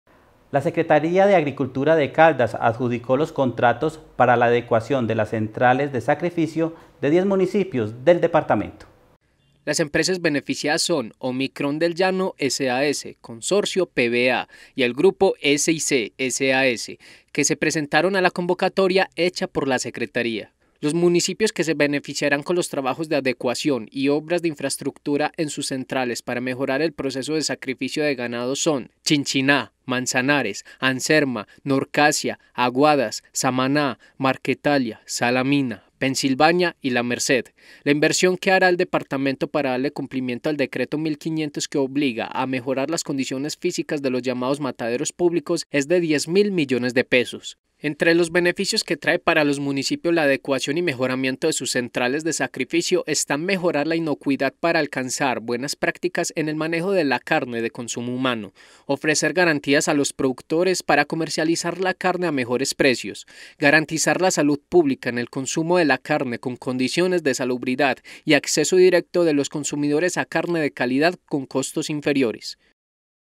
ADJUDICADAS OBRAS EN CENTRALES DE SACRIFICIO DE CALDAS
En audiencia pública la Secretaría de Agricultura de Caldas adjudicó los contratos para la adecuación de las centrales de sacrificio de diez municipios del departamento.